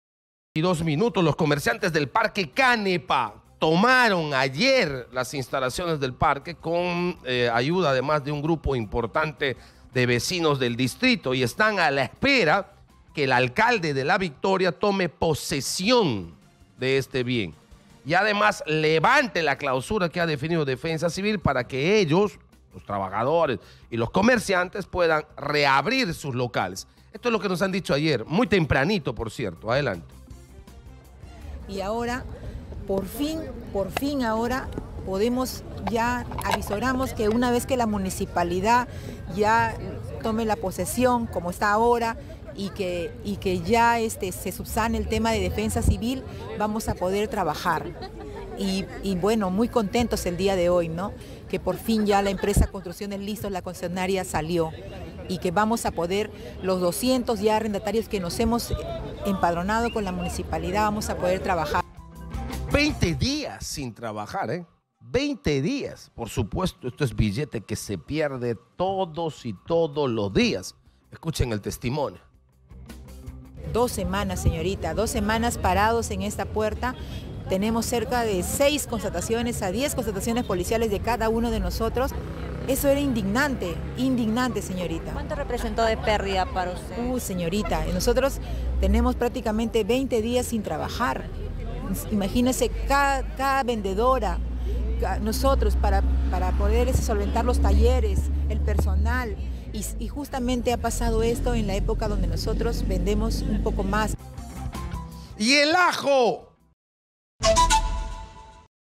🔴🔵Comerciantes del parque Cánepa reportan pérdidas económicas tras cierre de locales
Decenas de comerciantes irrumpieron en el centro comercial Parque Cánepa para exigir la devolución de la gestión del predio a la Municipalidad de La Victoria.

Exitosa Perú
"Exitosa: La voz que integra al Perú"

Corporación Universal